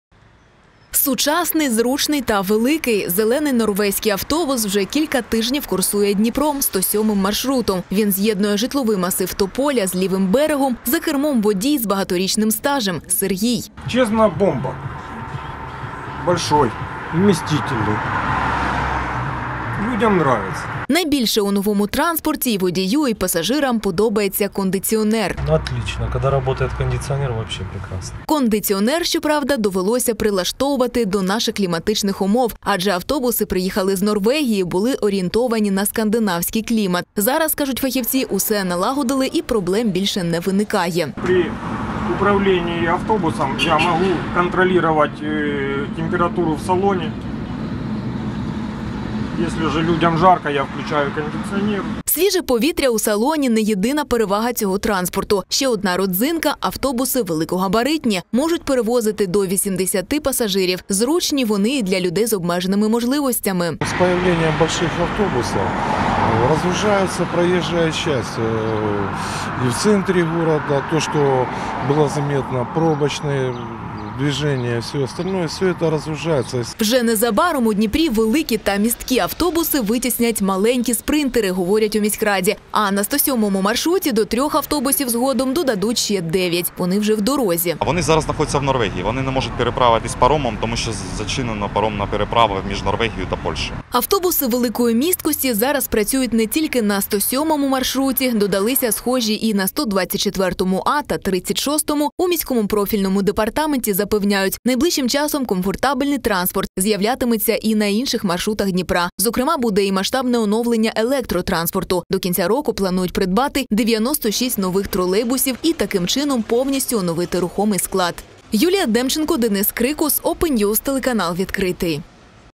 Замість маленьких автобусів - великі. Такі автобуси вже курсують 107-м маршрутом у Дніпрі
Замість малогабаритного, транспорт – великої місткості. Такі автобуси вже два тижні курсують 107-м маршрутом у Дніпрі. Поки що є три машини. Автобуси не нові, кажуть у міськраді, але у гарному стані. Їх придбали у Норвегії. Як оцінюють такий  транспорт пасажири та водії, - далі в сюжеті.